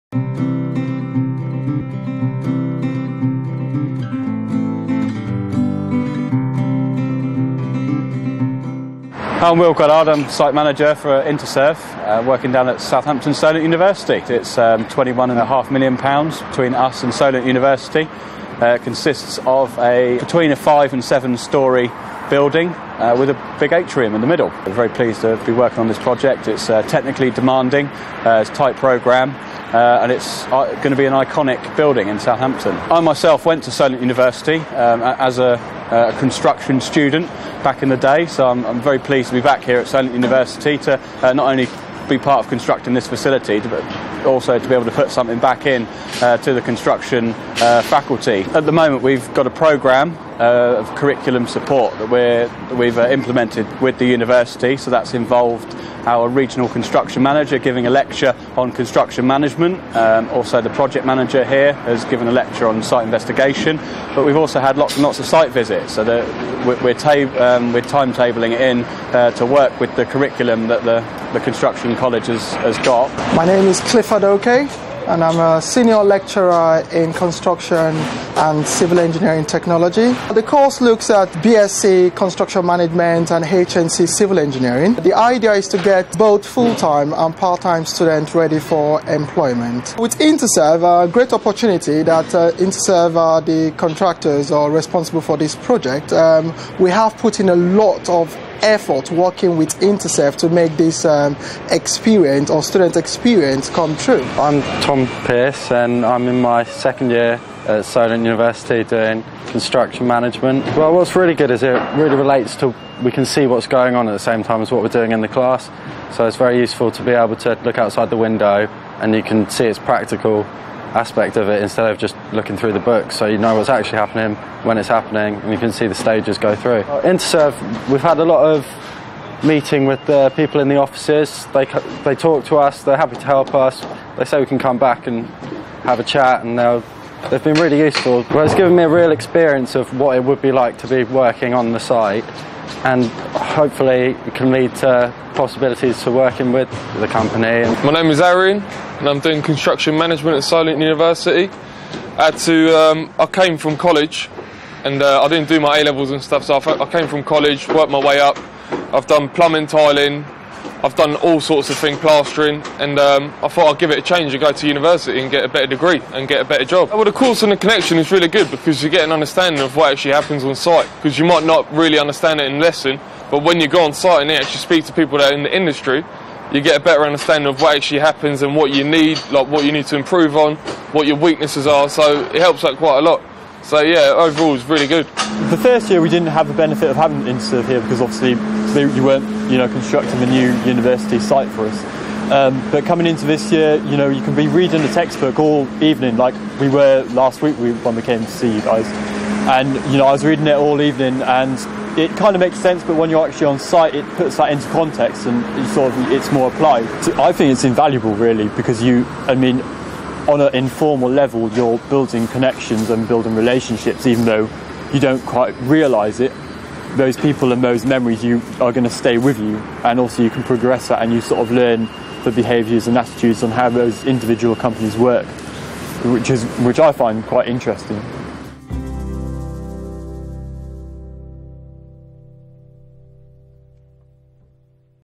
Southampton Solent University
Interserve's Construction division is constructing a £21.5 million 5 - 7 storey building at Southampton Solent University.

The architecture, including the central atrium, will make this an iconic structure in the city.